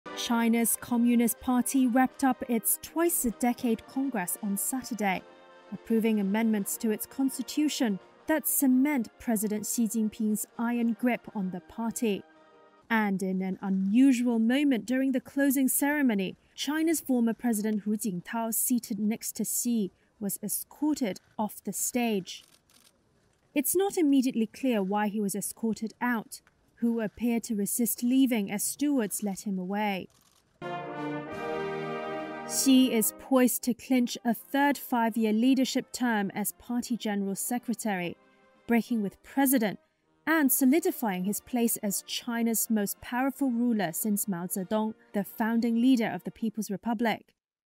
Chinese ex-President escorted out of party congress
China's Communist Party wrapped up its twice-a-decade congress. Hu Jintao was escorted out of the closing ceremony.